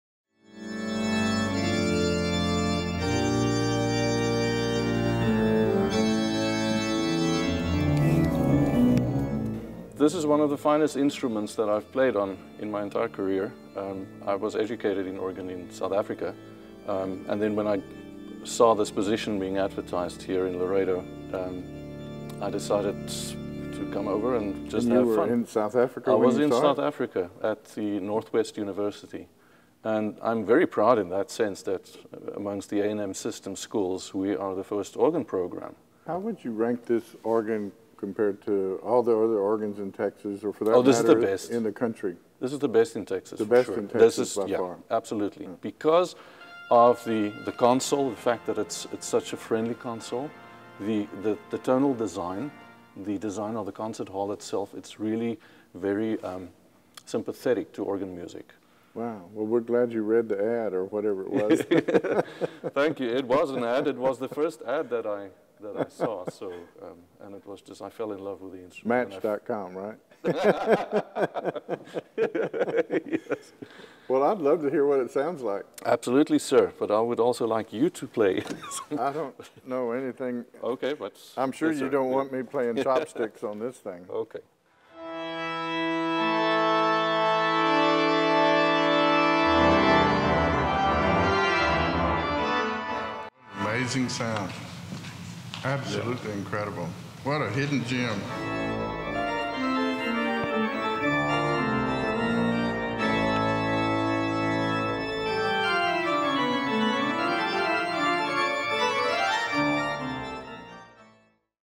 A Musical Treasure
THE SATISFACTION OF MUSIC: Dr. Colin Campbell gives Chancellor Sharp a lesson on the Sharkey-Corrigan Organ at Texas A&M International University.

The Sharkey-Corrigan Organ was gifted to the Recital Hall in 2006 by longtime Laredo businessman E. H. Corrigan. The organ is 69 ranks and 52 stops, and is built by the Kegg Organ Company.